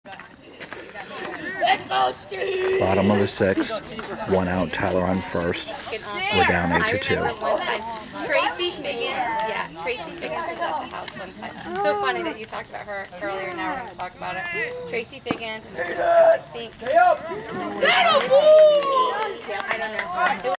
SnapDragon's Fall Baseball Gm-2 9/8/18 Pt 9/10
Bottom of the 6th, Tyler on 1B, 1 out, down 8-2. vs Cape Riptide